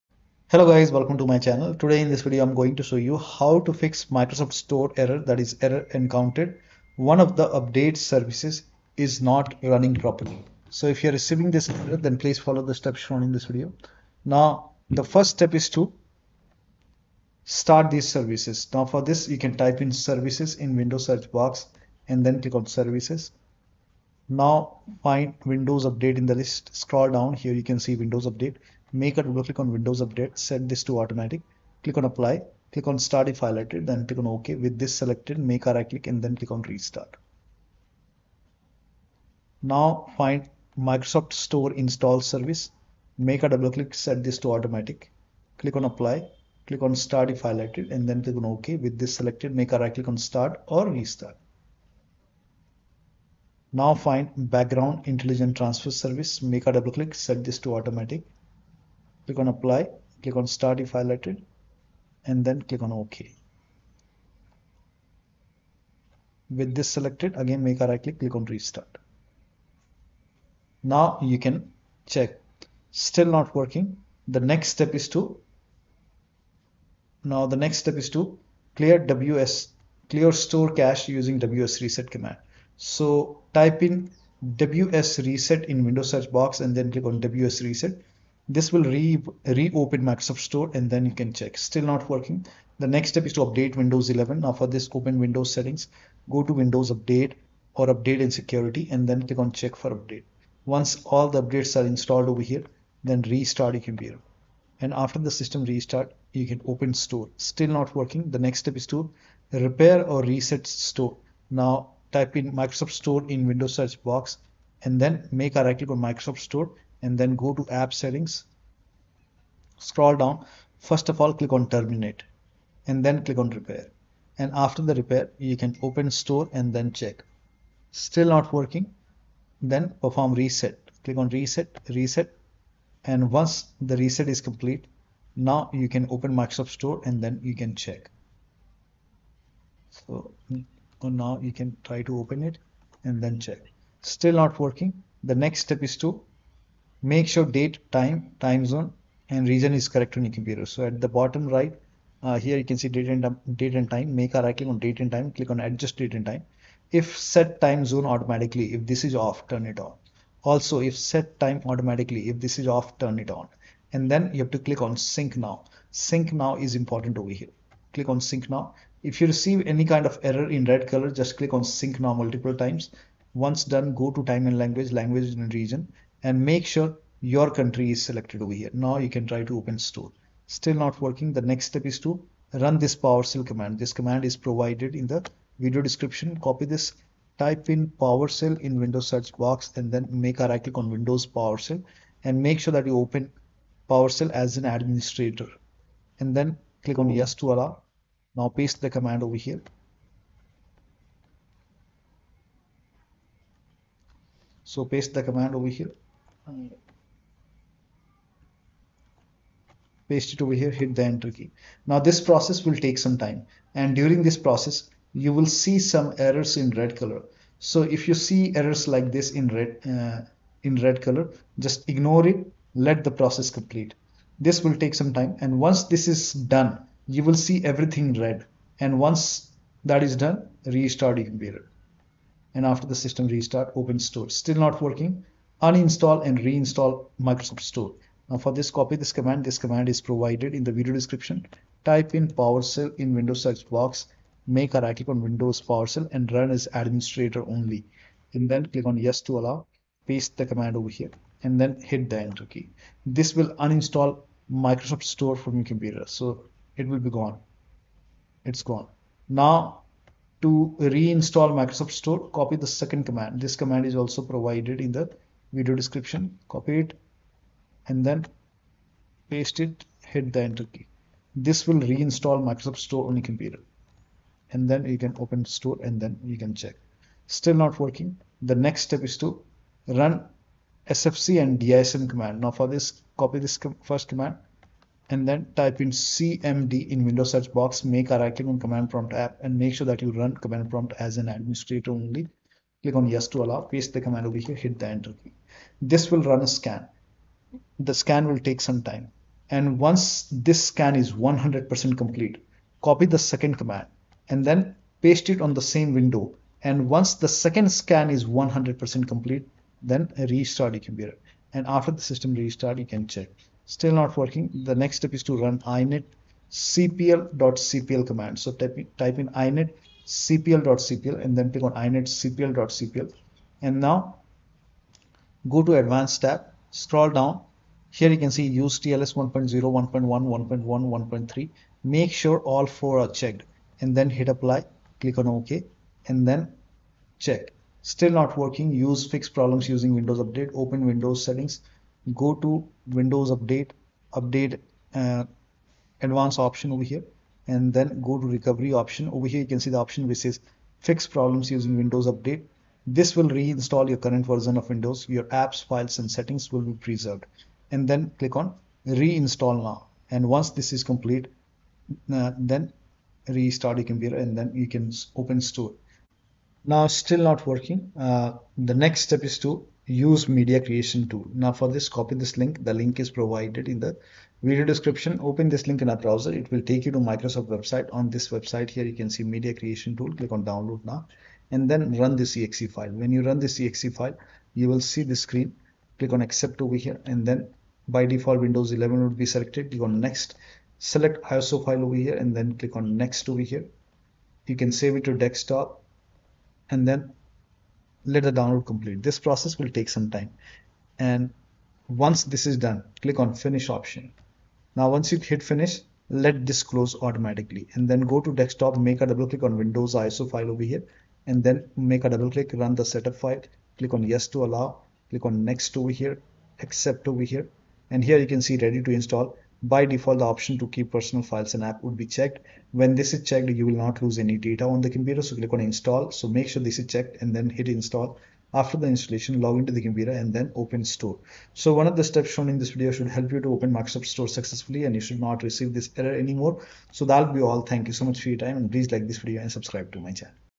Fix Microsoft Store Error Encountered One Of The Update Services Is Not Running Properly
How To Fix Microsoft Store Error Encountered One Of The Update Services Is Not Running Properly
Step 1) Start/Restart Windows Update, Microsoft Store Install service and background intelligent service
Step 2) Clear store cache Using wsreset command
Step 3) Update Windows 11
Step 4) Repair/Reset Store app
Step 5) Make Sure Date and Time, time zone, region are correct
Step 6) Run this Powershell command
Get-AppxPackage -AllUsers| Foreach {Add-AppxPackage -DisableDevelopmentMode -Register “$($_.InstallLocation)\AppXManifest.xml”}
Step 7) Uninstall & Reinstall Store
Get-AppxPackage -allusers *WindowsStore* | Remove-AppxPackage

Get-AppxPackage -allusers *WindowsStore* | Foreach {Add-AppxPackage -DisableDevelopmentMode -Register “$($_.InstallLocation)\AppXManifest.xml”}
Step 8) Run DISM Command
sfc /scannow
Dism /Online /Cleanup-Image /RestoreHealth
Step 9) Run inetcpl.cpl command
In Advanced Tab
Check Use TLS 1.0, 1.1, 1.2 and 1.3
Step 12) Reset Windows 11 (Create a backup first as it may result in data loss)